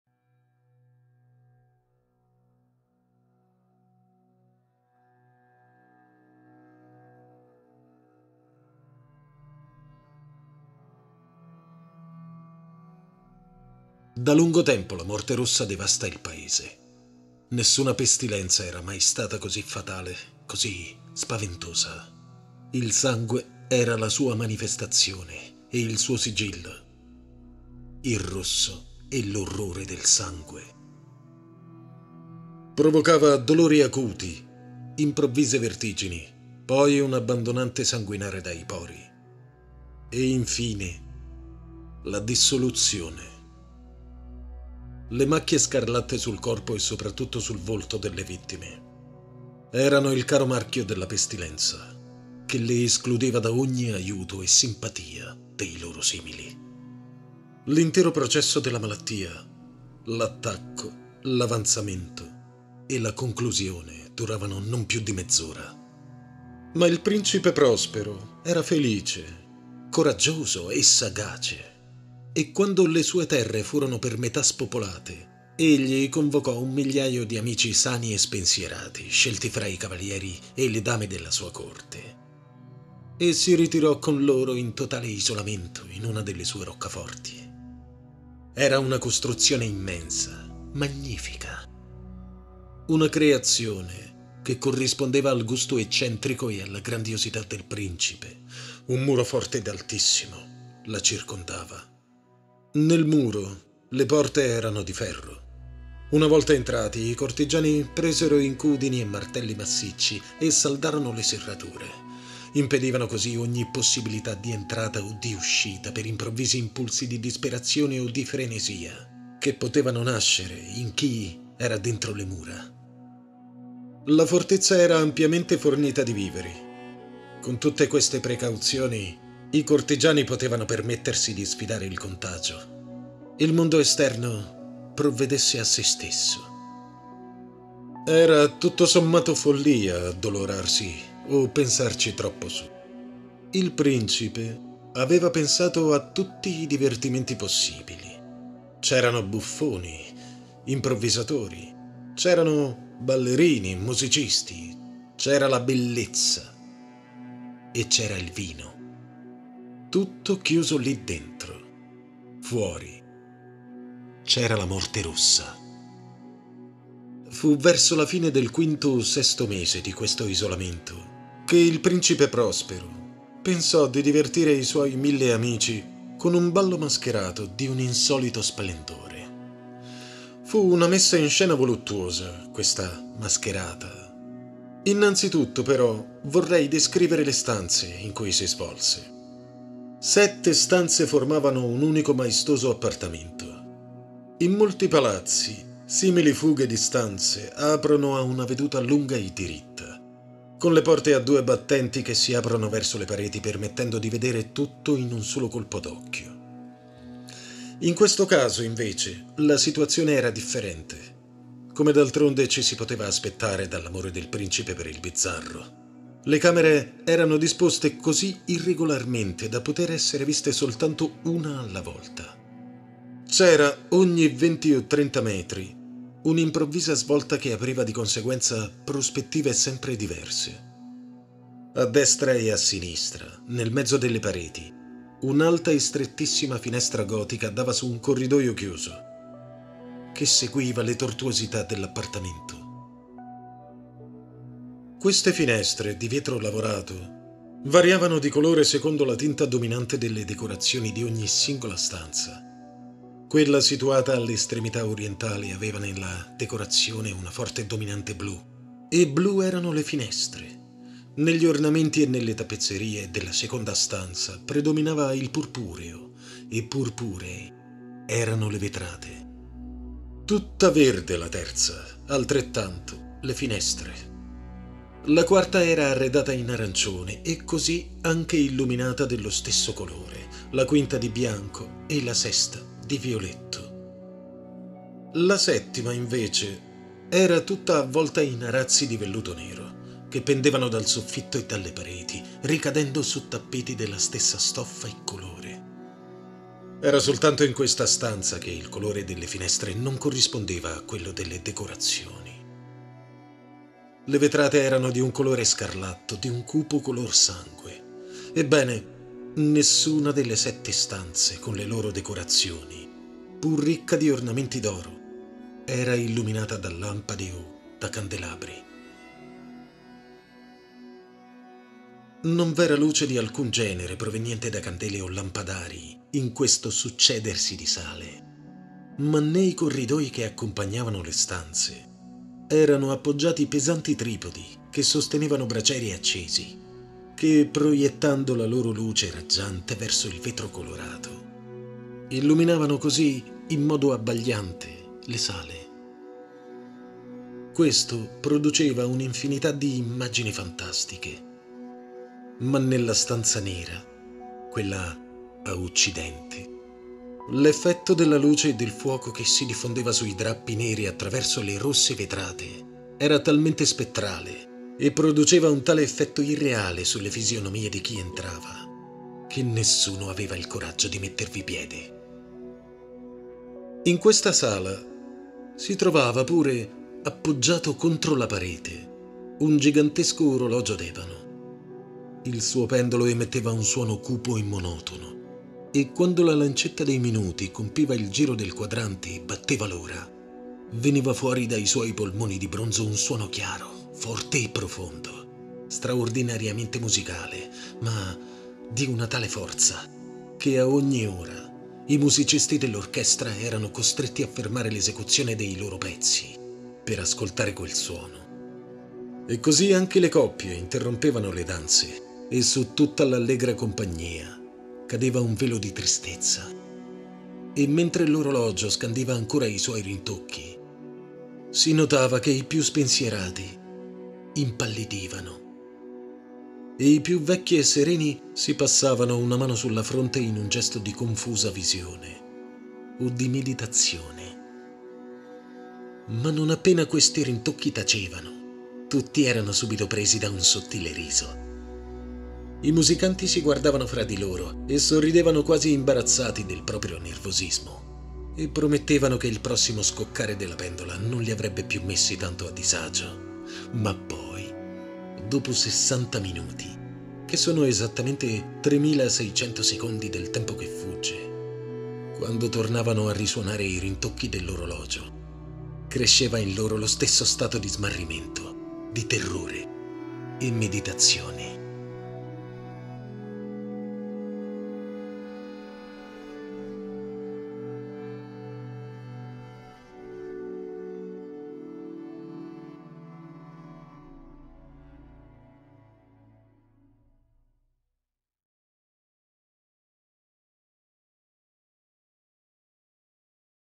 AUDIOLIBRO - La mascherata della morte rossa (I racconti del terrore, di Edgar Alla Poe)
The Masque of the Red Death è un racconto di Edgar Allan Poe, pubblicato nel 1842. Una terribile pestilenza, la Morte Rossa, sta devastando una contrada..

Favole Per Bambini | Storie Per Bambini | Storie italiane | Fiabe Per Bambini | Favole Per Bambini Raccontate | Fiabe Italiane | Italian Fairy Tales